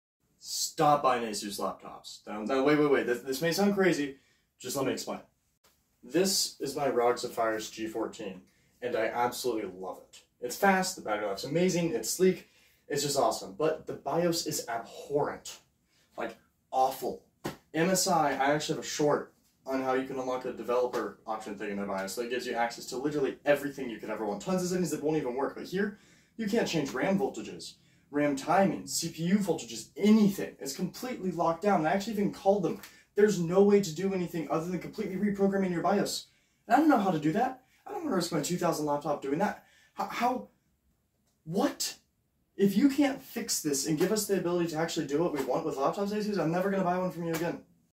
STOP BUYING ASUS LAPTOPS!!!!
Cmon Asus, it's not that hard. We all know you already have a developer BIOS you use in-house.